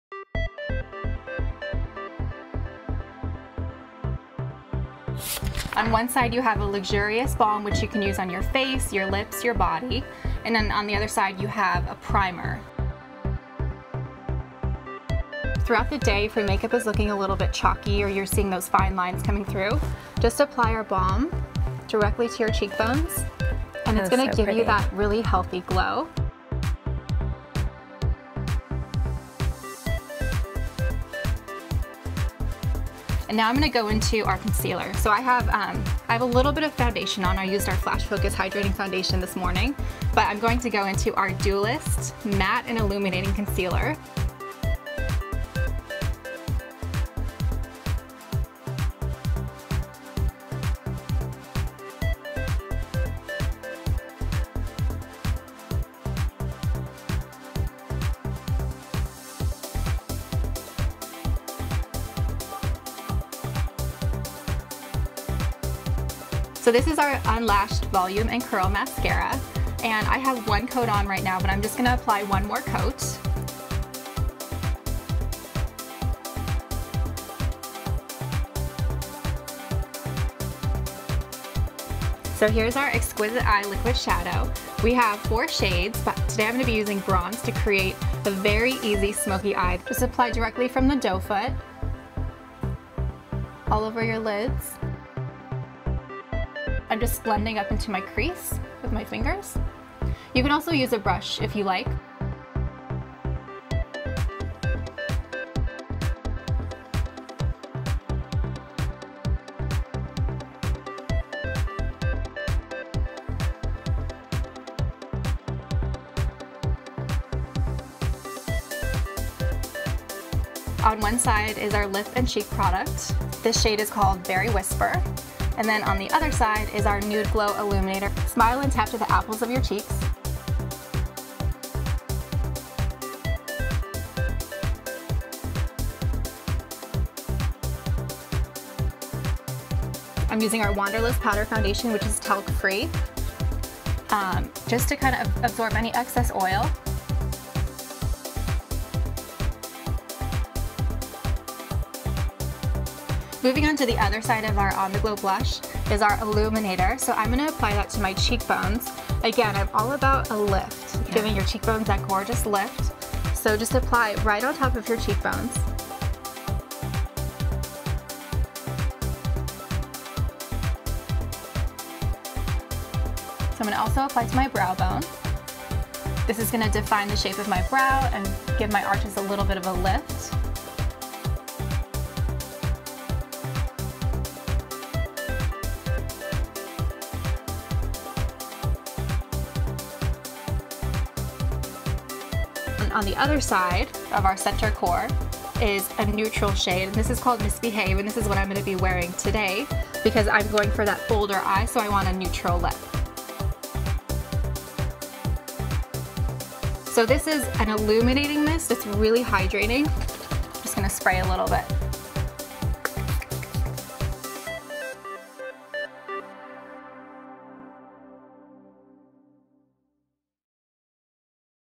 Lindsay Ellingson's Date Night Look
Ever wonder what a supermodel wears for makeup? We do. That’s why we asked model and Wander Beauty co-founder Lindsay Ellingson to show us her favorite products for a pretty date night makeup look.